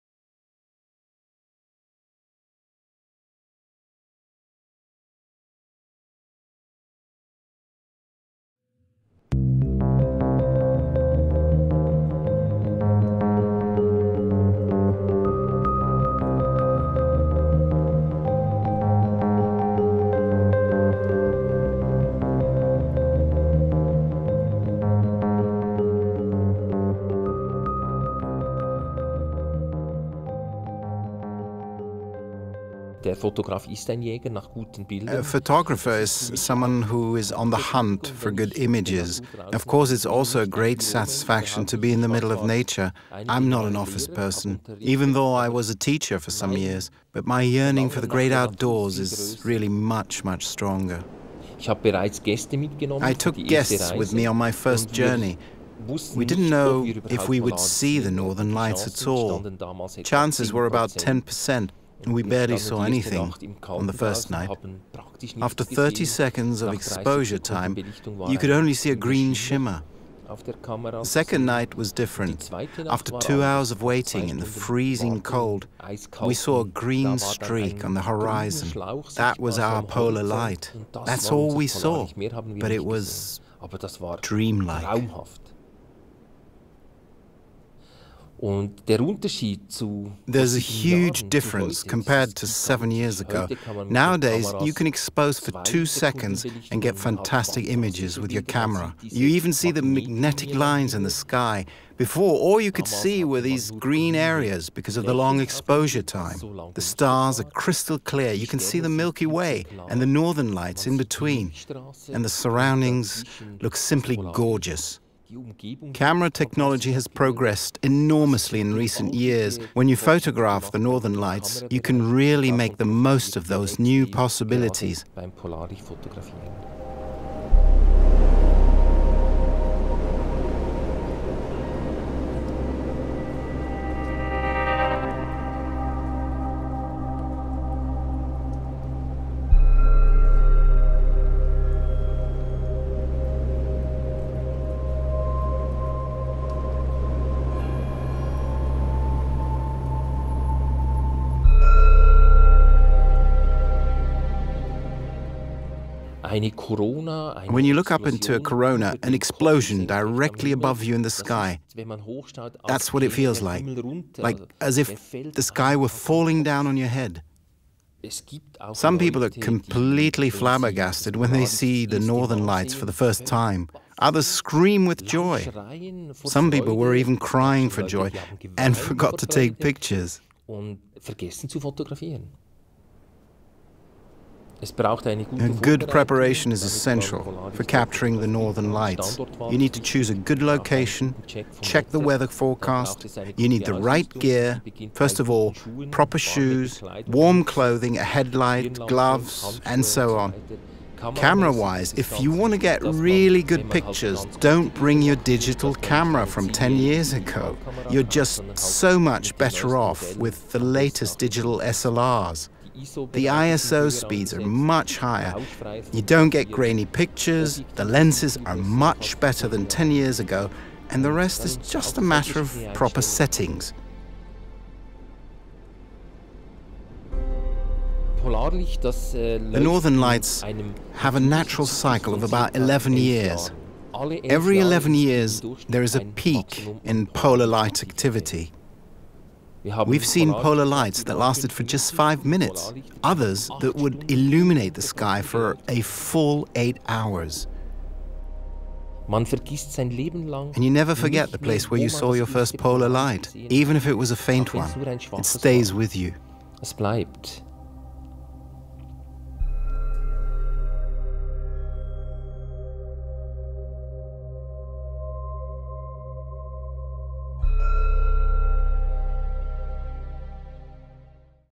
Shooting the Northern Lights with Nikon pro Dionys Moser | Photographers' Voice series
Hand in camera and waiting in subzero temperatures, the once teacher, but now Nikon Pro photographer, Dionys Moser, shares what it means to chase the Northern Lights. Aurora Borealis, the scientific term for this fascinating natural phenomenon, is a much sought after destination for keen and pro photographers alike. Shot around Tromsø, in Norway. Watch, pick up some tips and be inspired to do more.